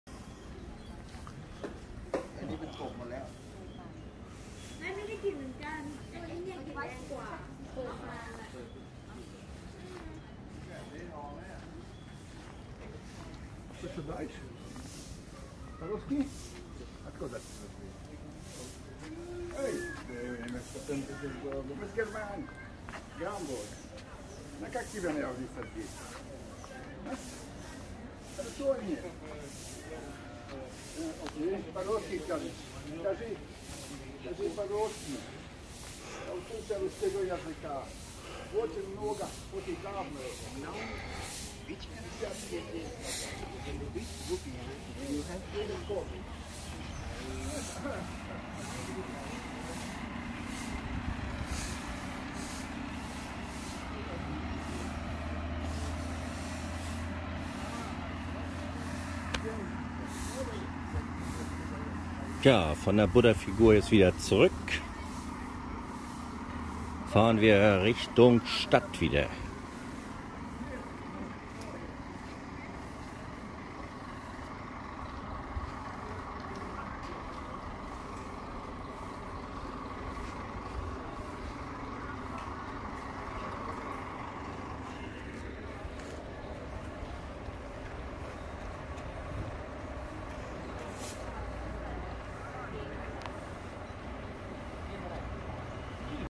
Big Buddha Pattaya
Der goldene Big Buddha hoch über Pattaya